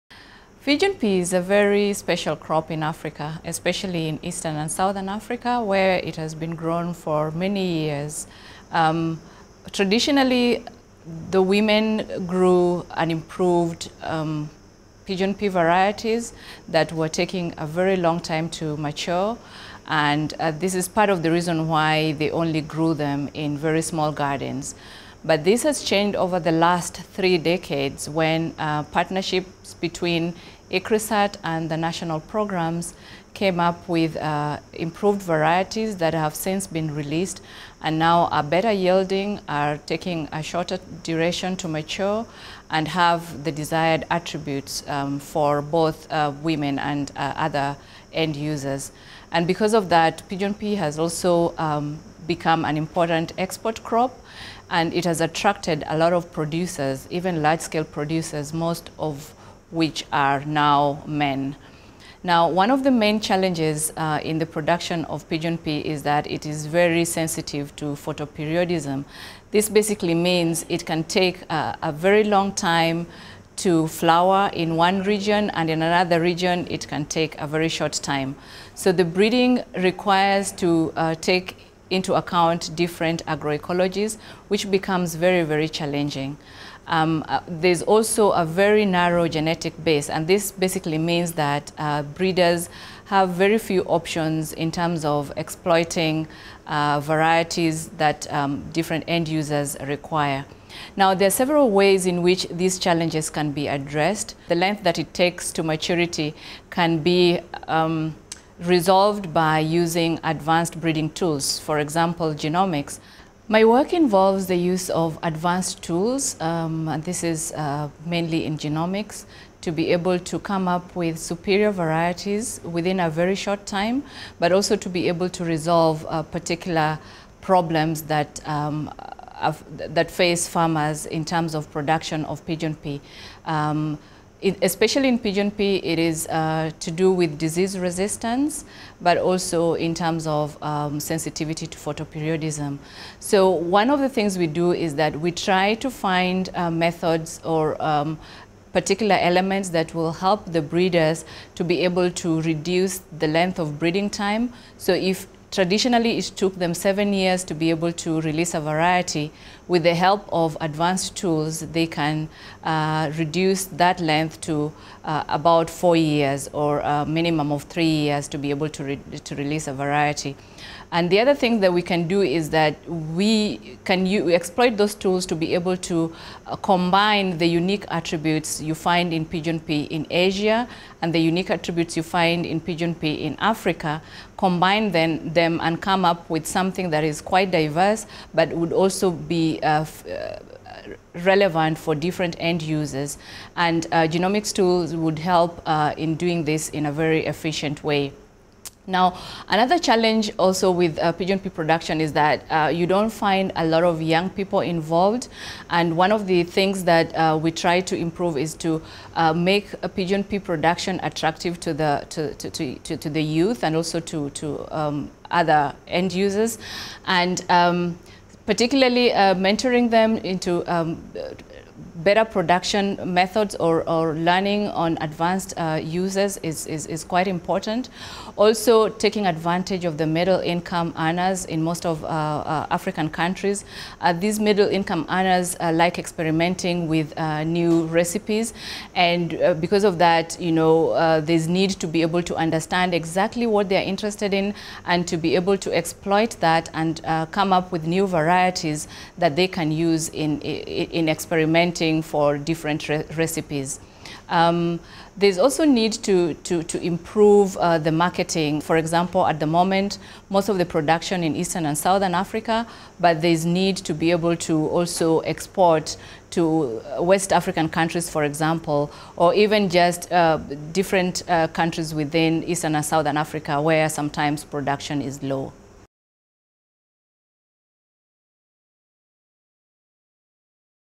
Pigeonpea breeding challenges in East and South Africa: Genomics the way out
The challenges to breeding pigeonpea in East and South Africa can be surmounted by the use of advanced tools such as genomics, combining the crop’s unique attributes from different regions, mentoring youth, and exploiting export markets within Africa.
 - Dr Damaris A Odeny, Scientist – Biotechnology (ESA), Genomics & Trait Discovery, Genetic Gains Program, International Crops Research Institute for the Semi-Arid Tropics (ICRISAT).